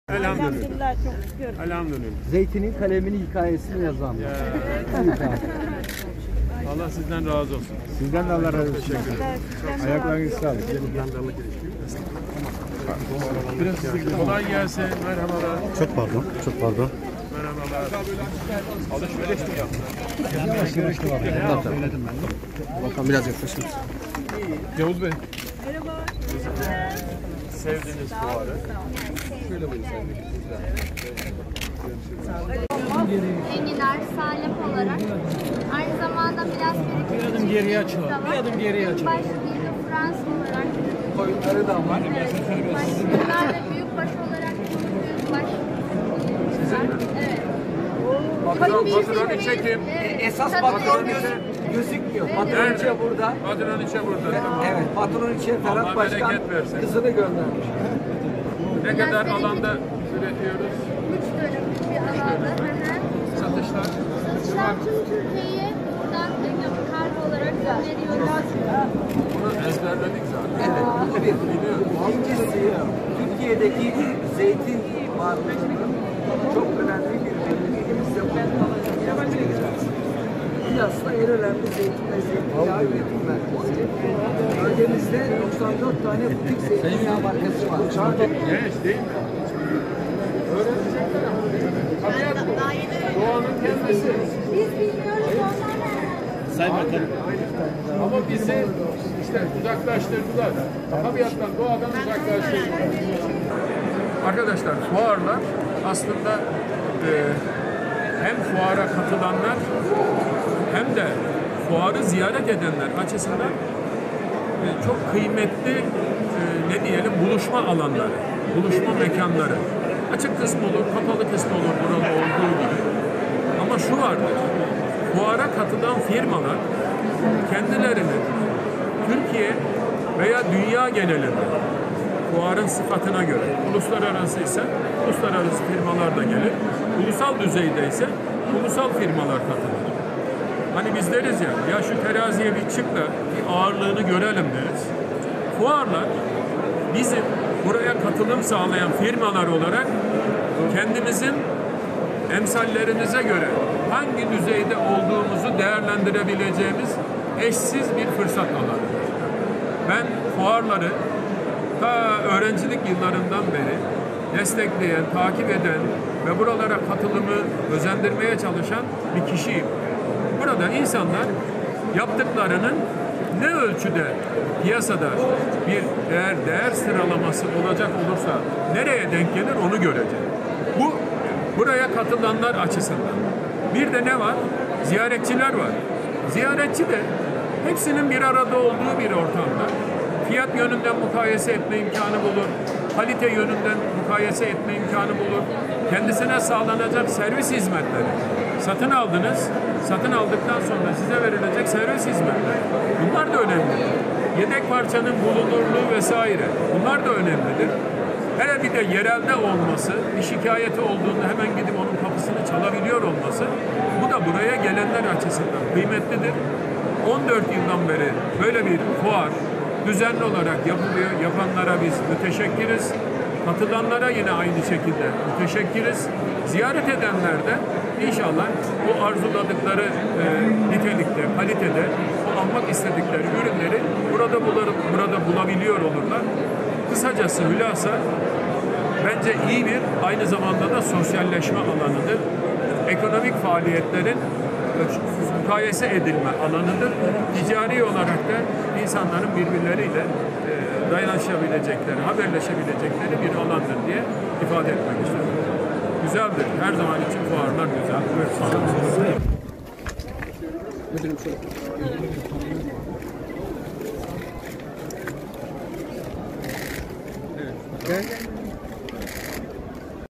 Bakan Kirişci tarım fuarına katıldı
Tarım ve Orman Bakanı Prof. Dr. Vahit Kirişci, Muğla'nın Milas ilçesindeki '14. Güney Ege Gıda, Tarım ve Hayvancılık Fuarı'nı ziyaret ederek sektör temsilcileri ve üreticiler ile buluştu